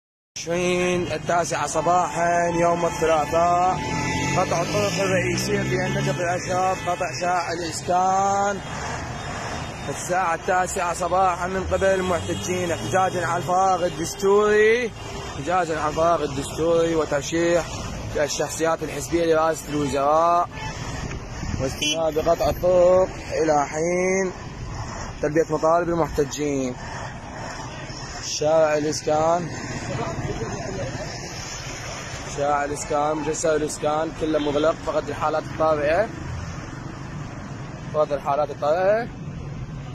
متظاهرو النجف يغلقون مجسر الإسكان وبعض الشوارع الرئيسة في المحافظة
ــــــــــــــــــــــــــــــــ